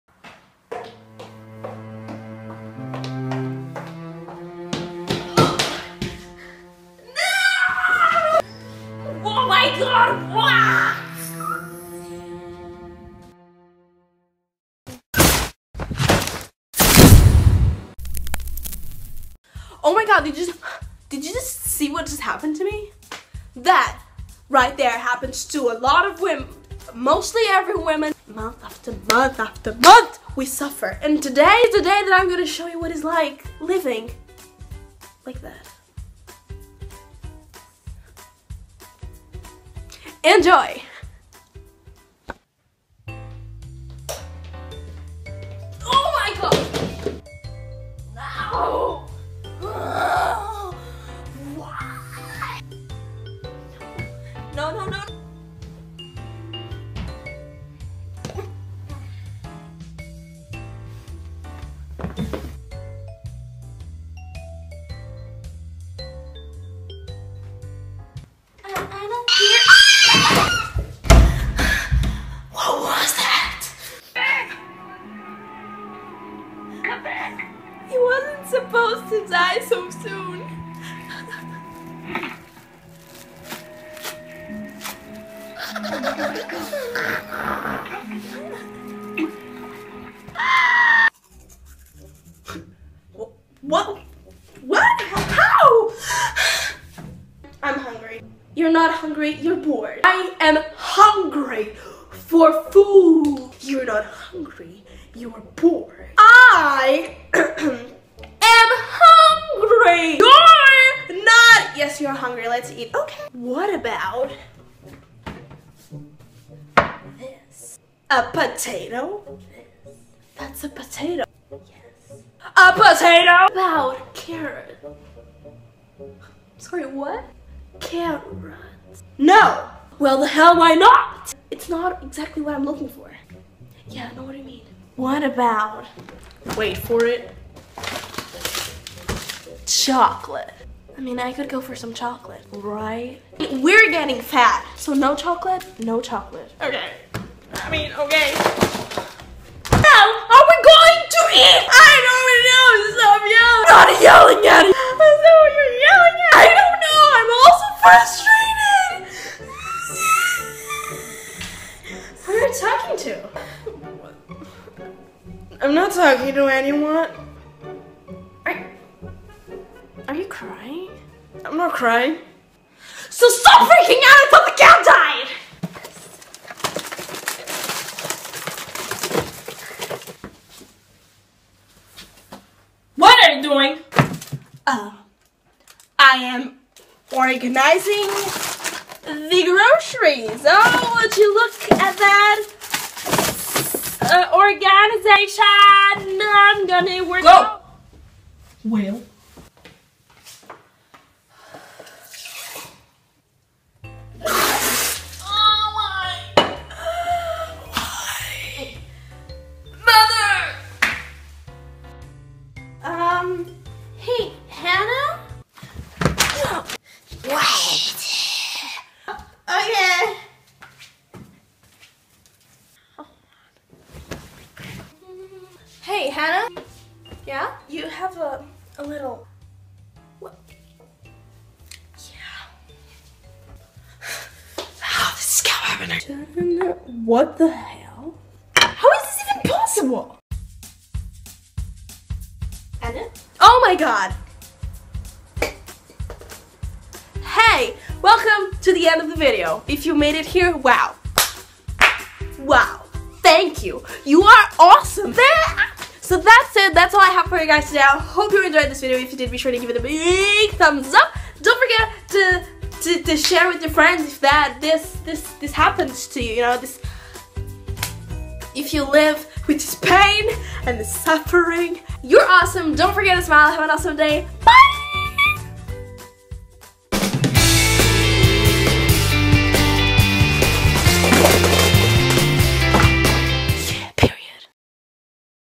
HOW GIRLS ACT ON THEIR PERIOD
My ovaries have been shot, THEY ARE BLEEDING, and that kids is how girls have their period! Hope you guys enjoy this video have an AWESOME DAY DON'T FORGET TO SMILE cya next time! Peace out, love you :)